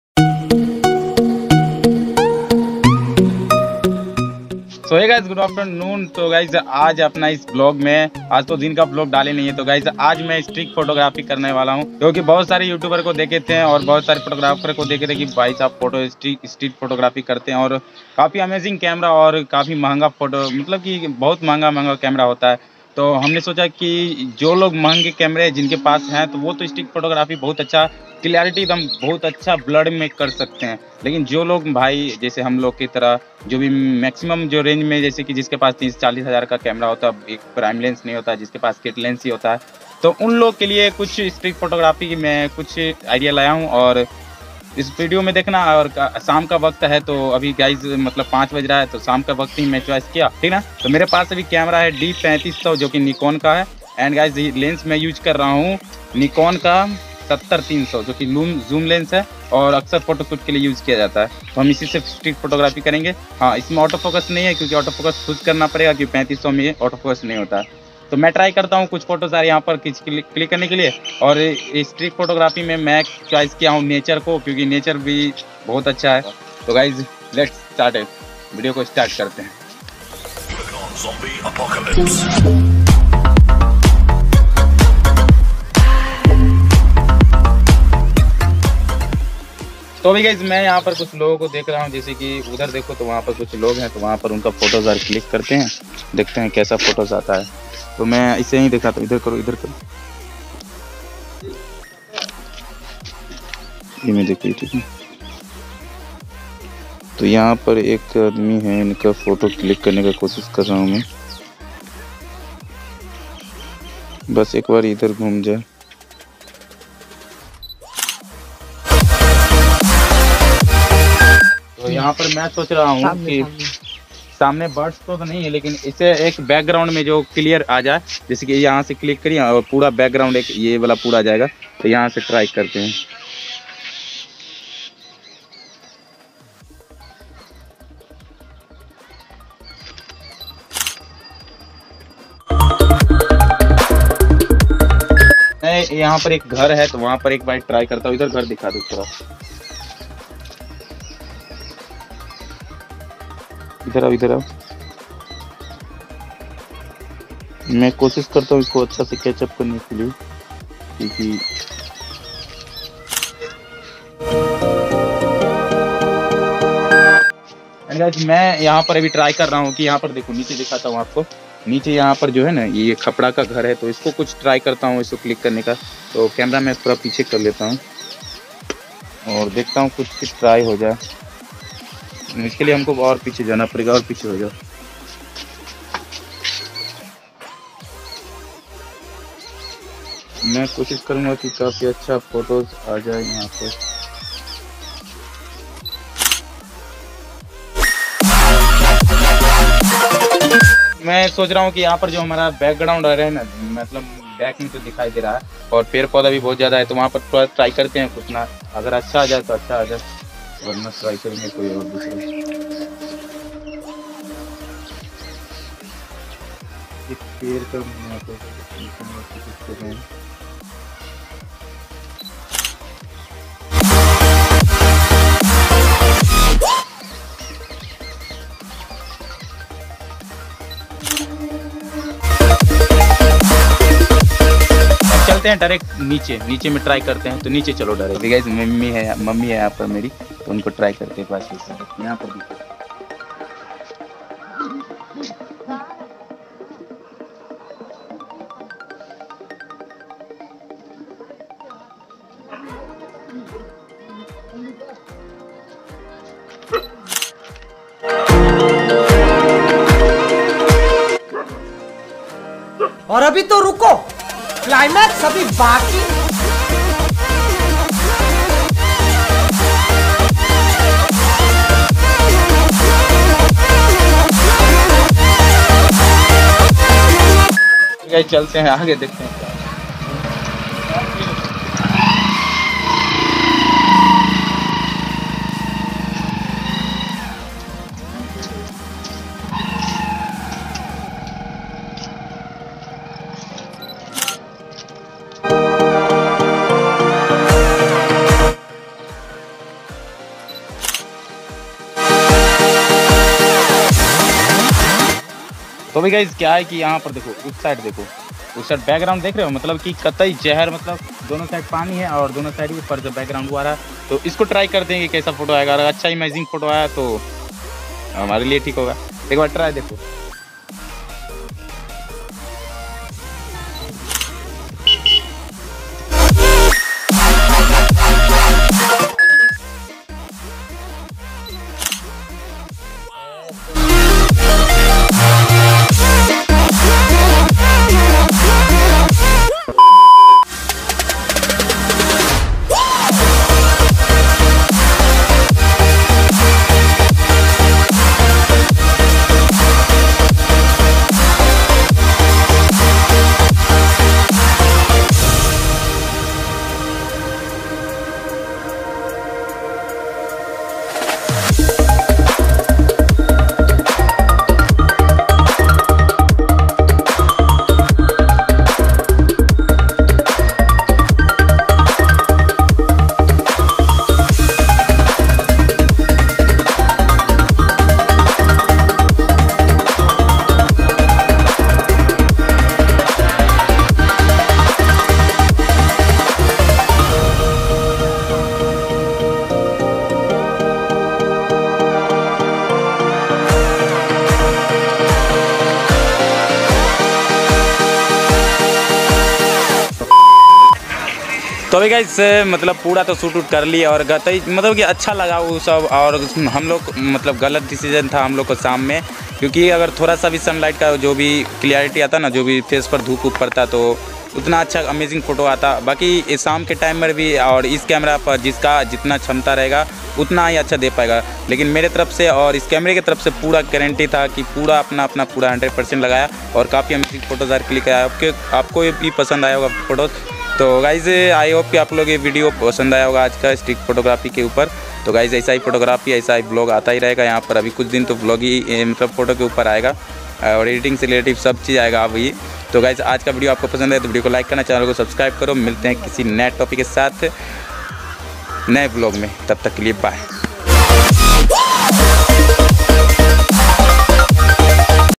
Street photography in village 📸 | Nicon d3500+nicon zoom lence 70-300
Hallo friends

This is my first Street photography ☺️

I hope you injoy this video

                                    Shere

                                                Subscrib